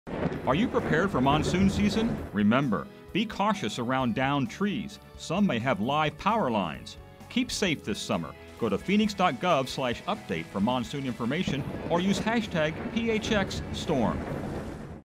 Monsoon Safety Tip #4 | Downed Trees are Dangerous
Be cautious around downed trees some may contain downed power lines. Be safe this summer! or if you are on social media use
Connect to the City of Phoenix on Facebook, Twitter and YouTube: